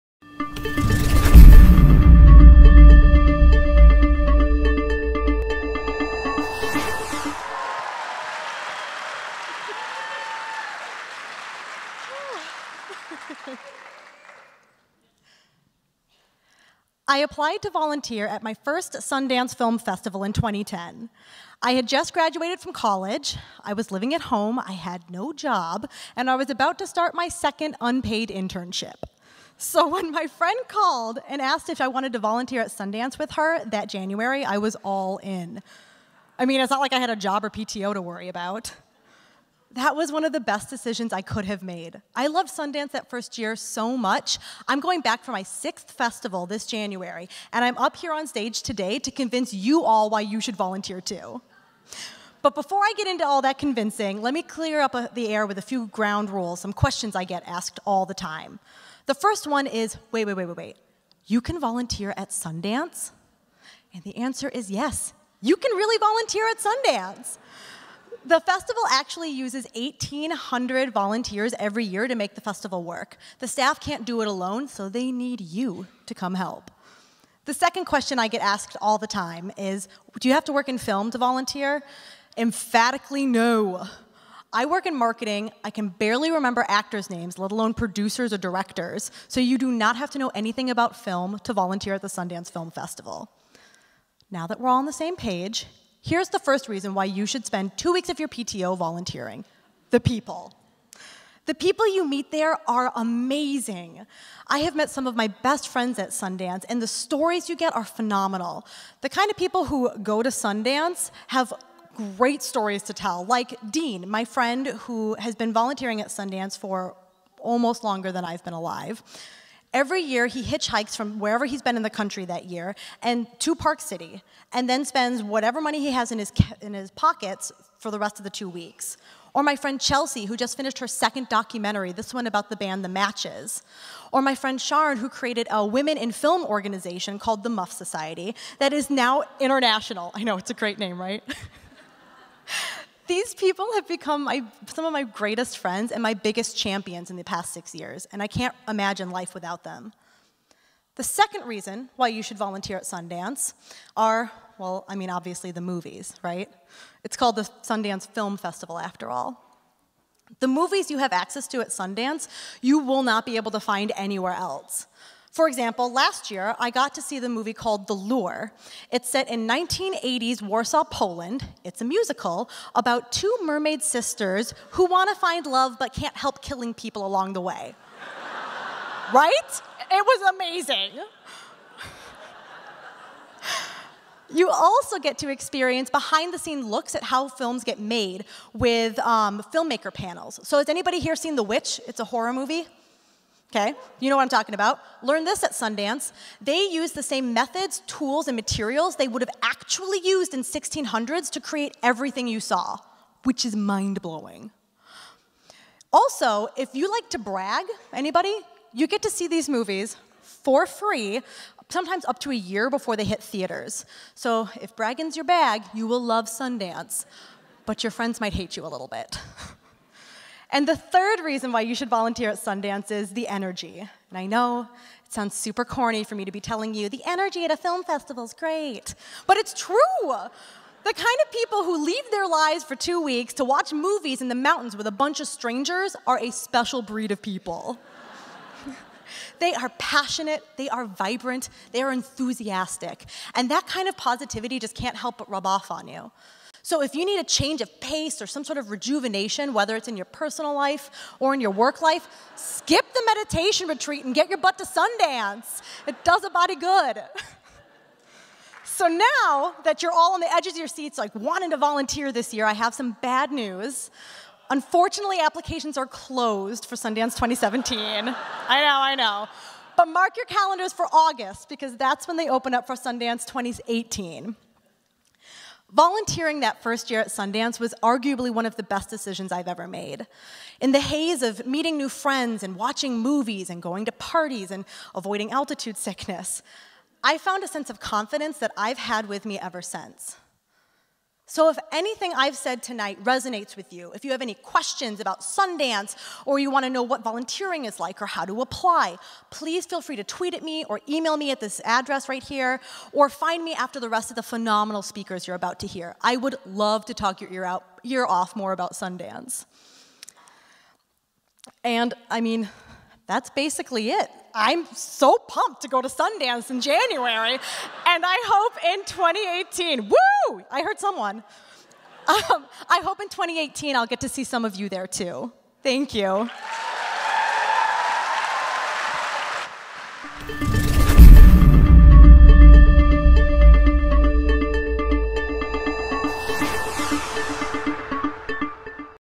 Katie Pennell – Why You Should Volunteer at Sundance
Katie Pennell giving her talk," Why You Should Volunteer at Sundance" at Ignite Minneapolis 11 on November 3, 2016 at the Ted Mann Concert Hall in Minneapolis.